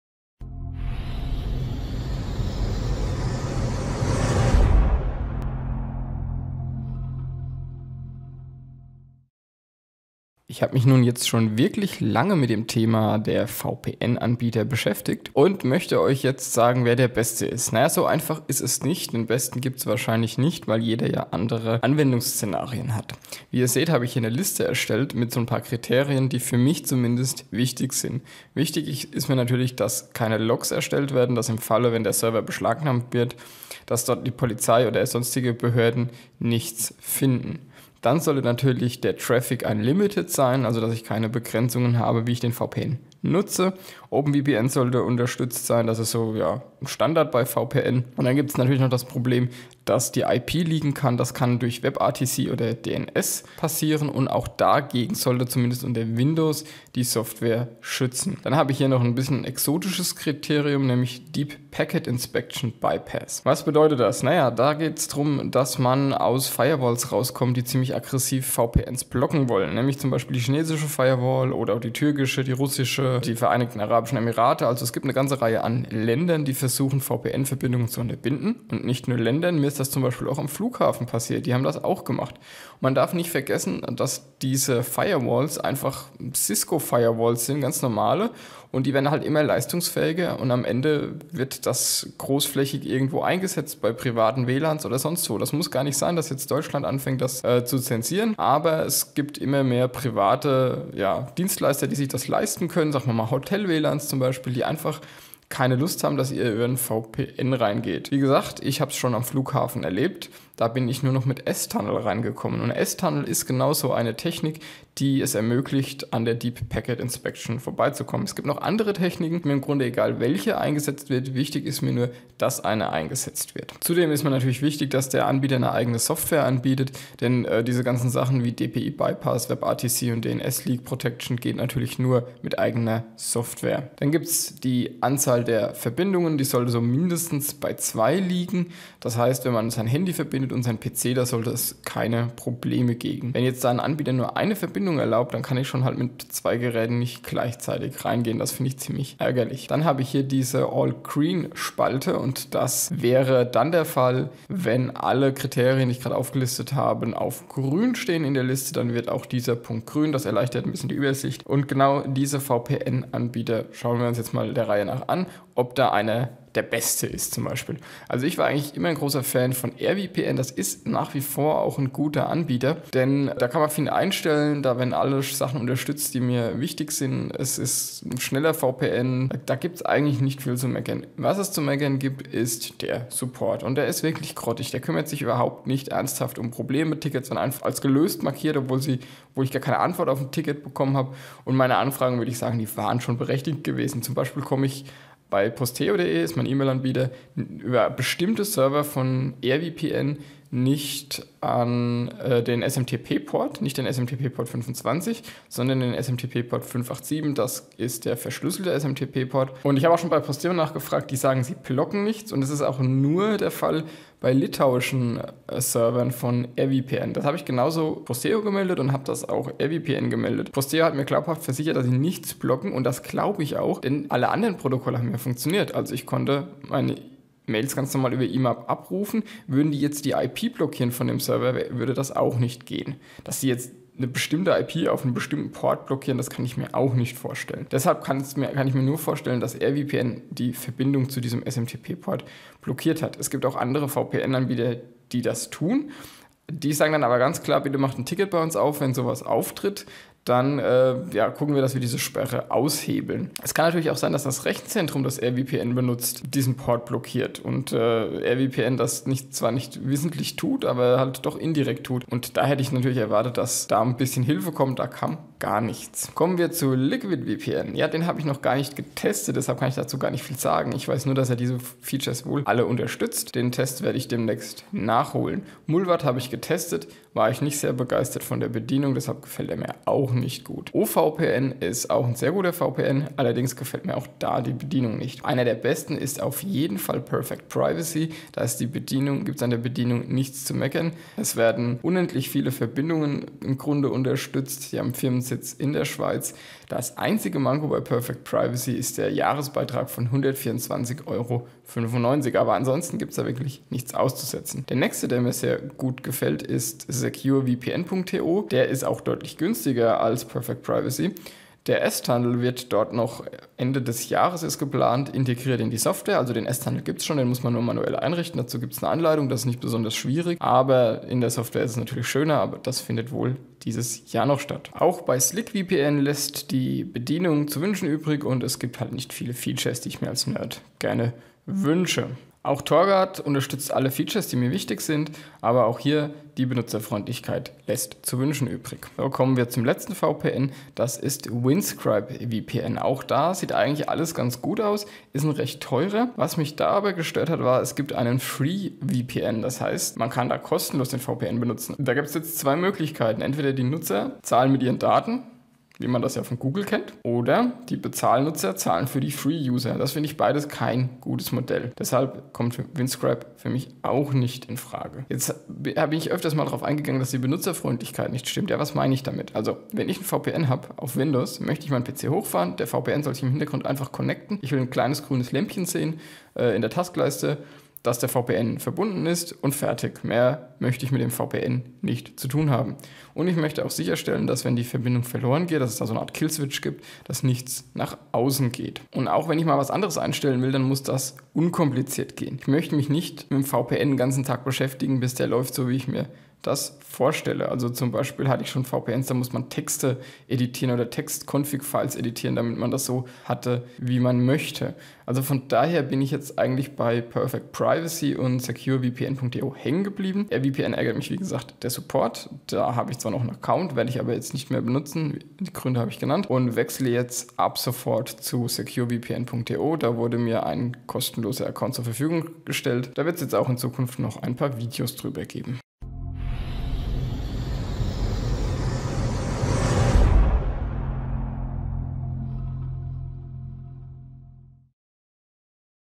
[Empfehlung] Der beste VPN 2017
Gibt es den besten VPN? Es gibt zumindest Kriterien dafür
Meine persönliche Empfehlung: